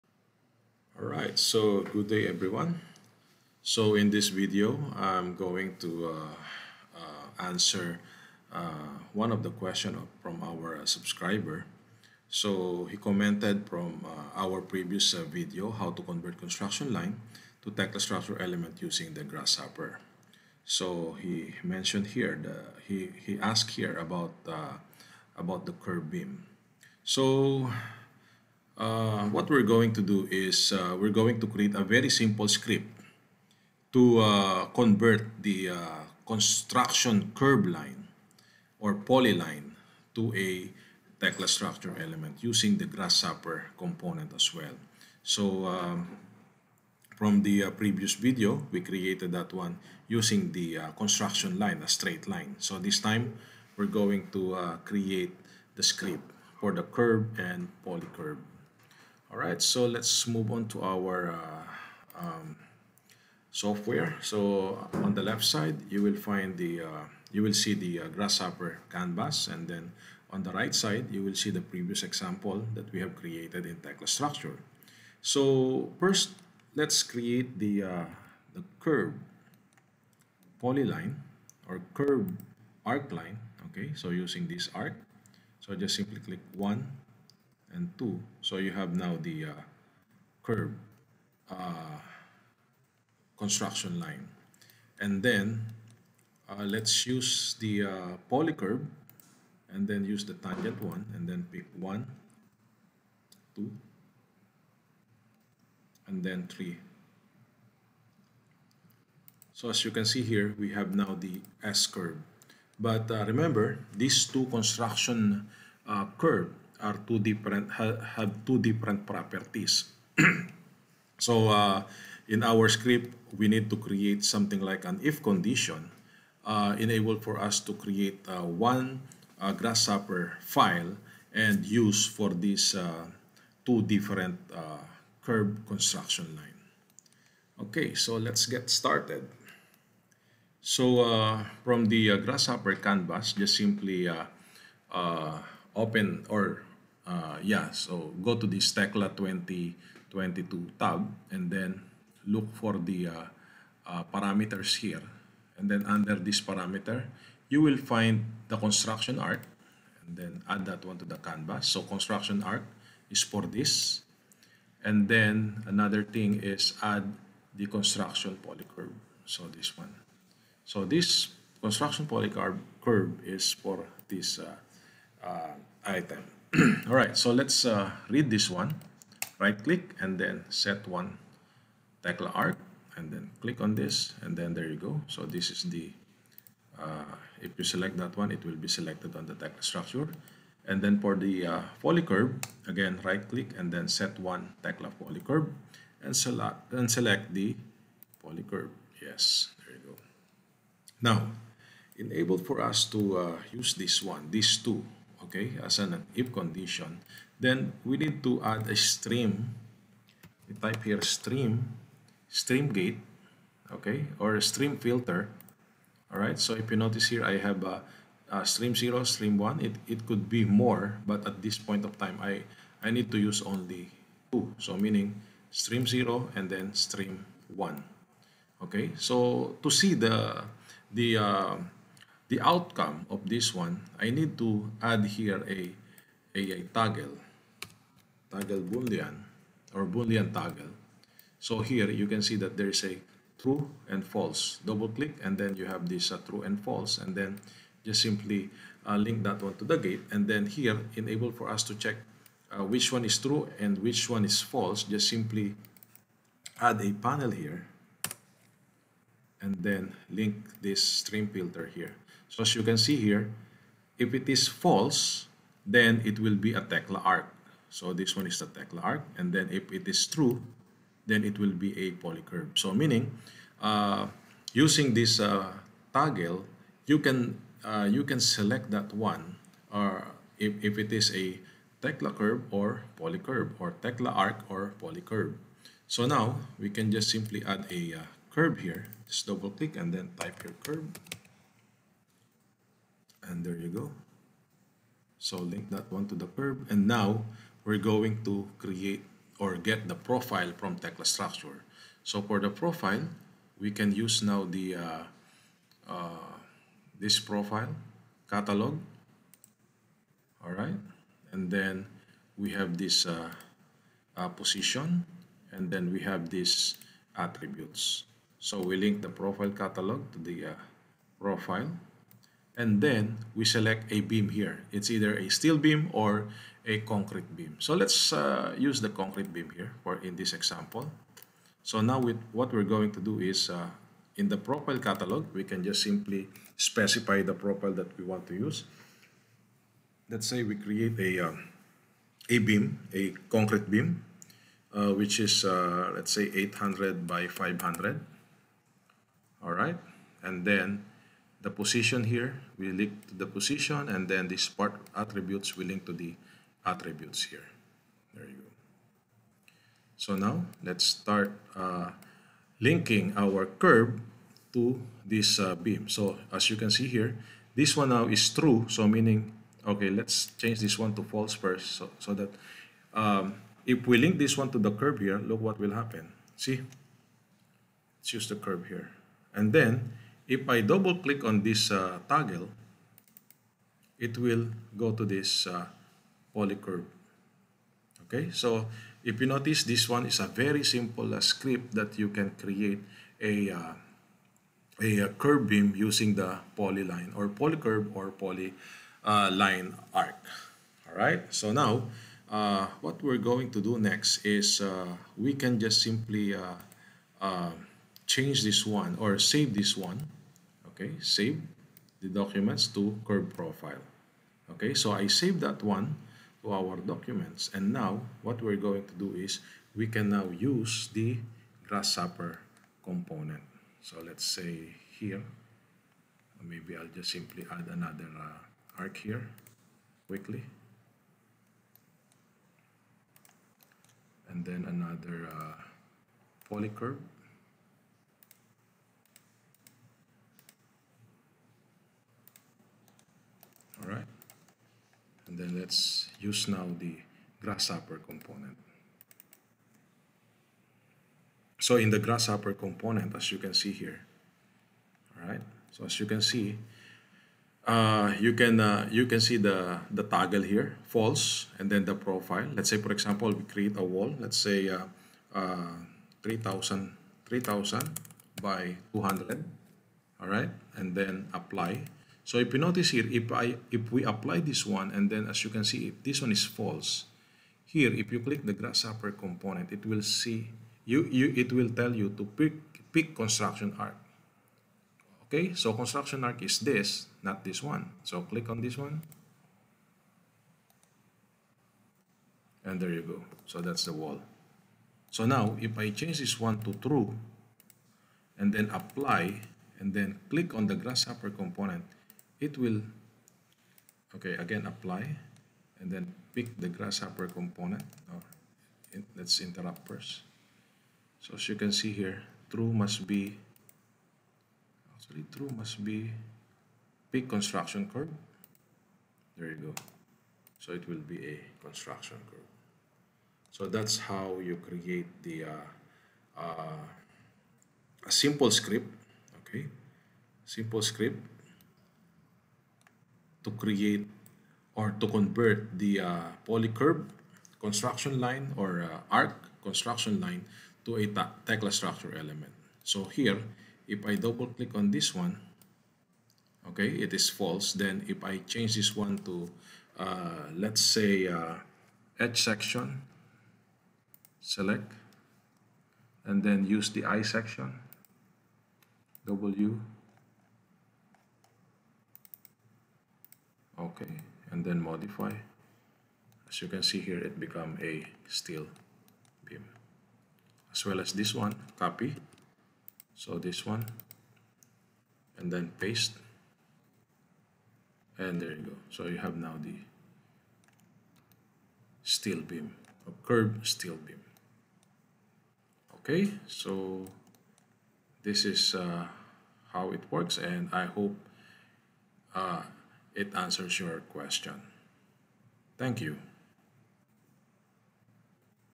How to convert Curve Construction line to Tekla elements using Grasshopper Component?
This video shows how to create a simple script for grasshopper component.
 It can be applied for both Steel and Precast Structures.

if you have a video tutorial request on HOW to do it in Tekla Structures, please feel free to send email to .